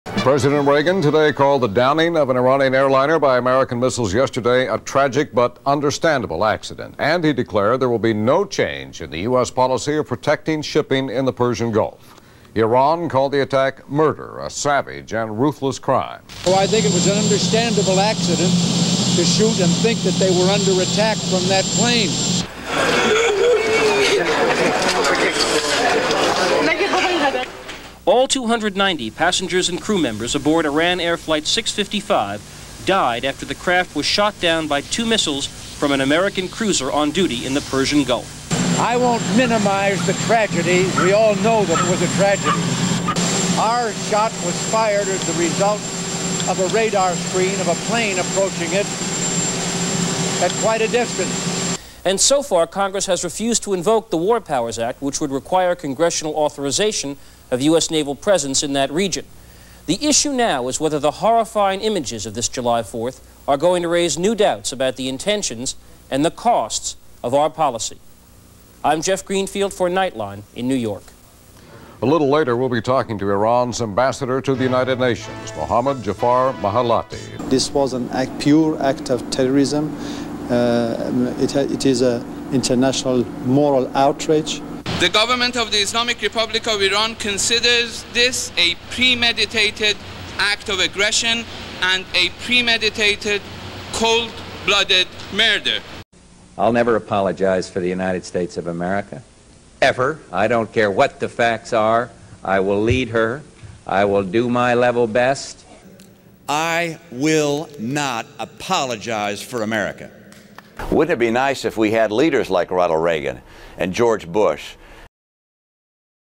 Reagan shot down Flight 655 (civilians)
"'Our shot was fired as a result of a radar screen and a plane approaching it at quite a distance."
- Ronald Reagan (1988)

Learn more about Iran Air Flight 655 by using Google search!